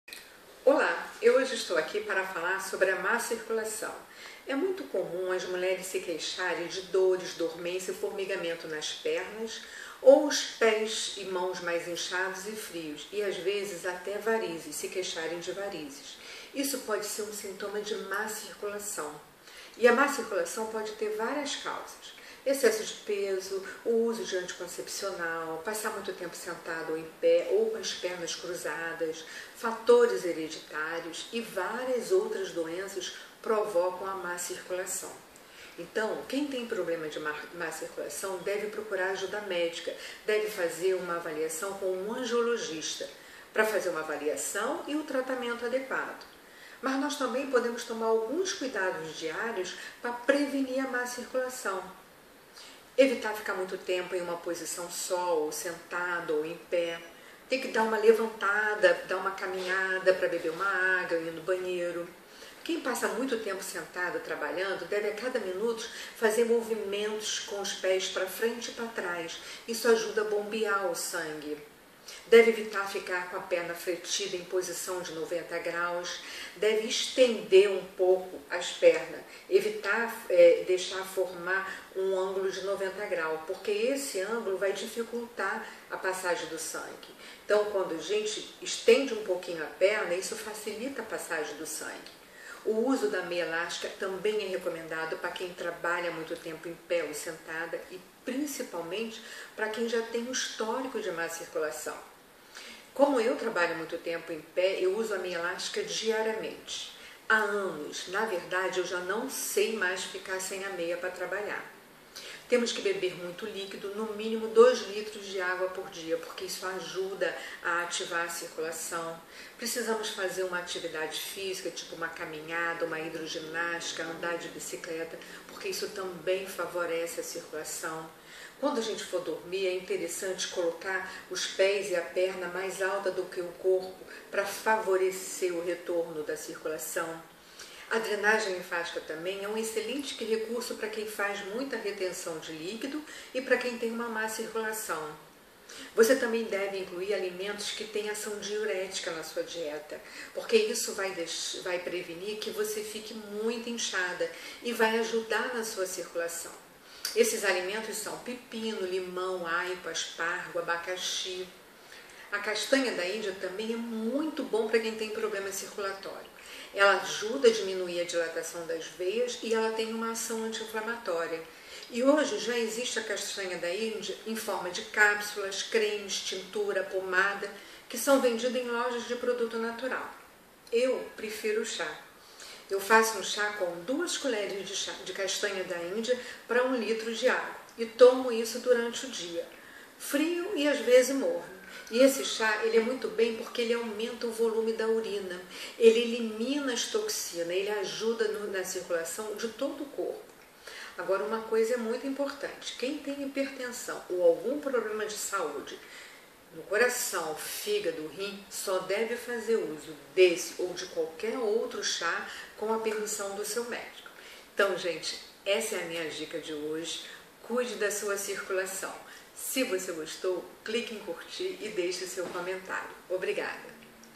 MÁ CIRCULAÇÃO
Os sintomas da má circulação são: dores,dormências e formigamentos nas pernas, pés e mãos inchados e frios e varizes.
Saiba o que fazer para minimizar e prevenir a má circulação.